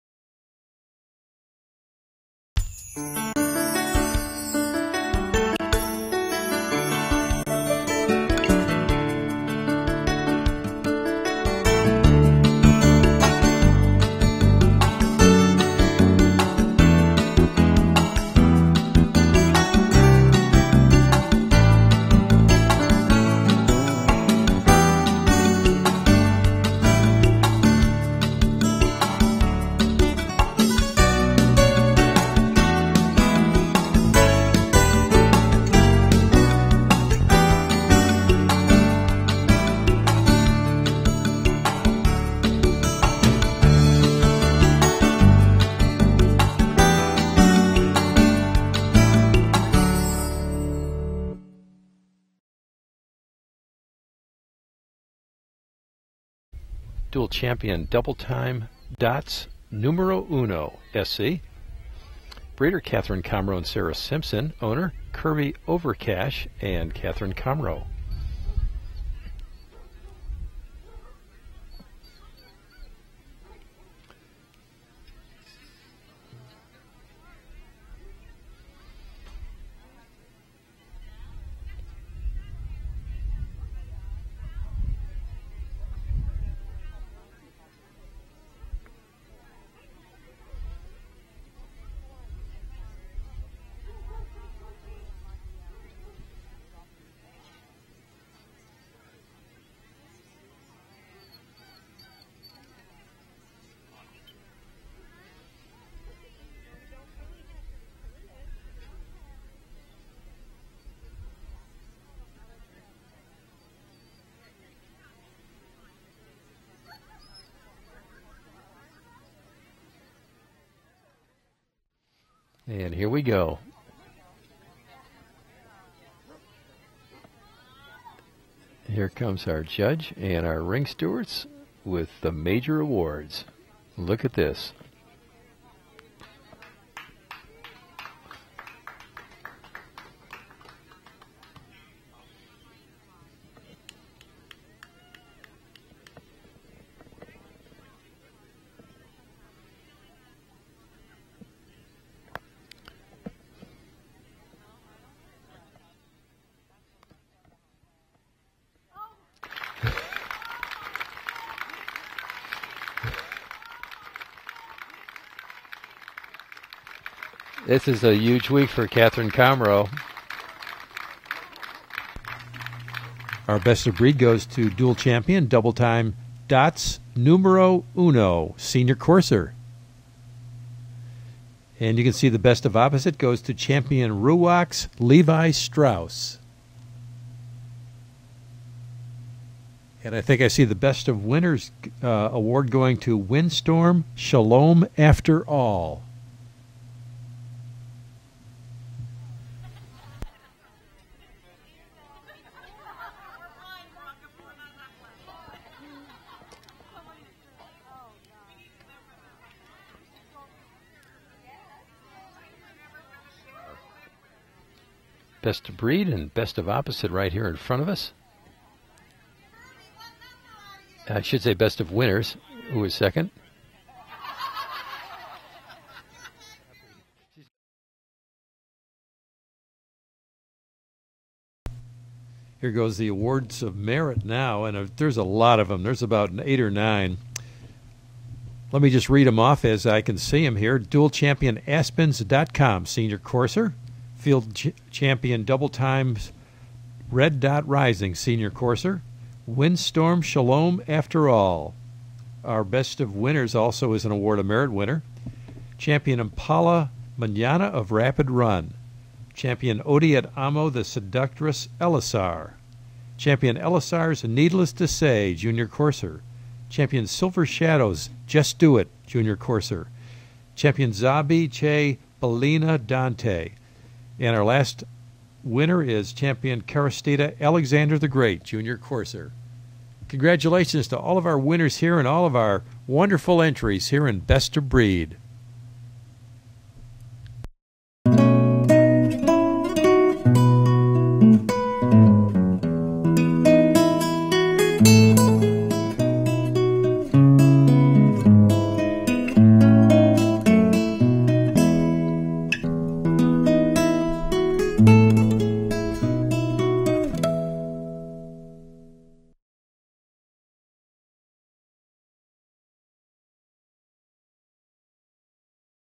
2008 Saluki Club of America Best of Breed
With nearly 100 AKC Champions of Record entered in Best of Breed, the SCOA National Specialty held in Lexington Kentucky was another huge success.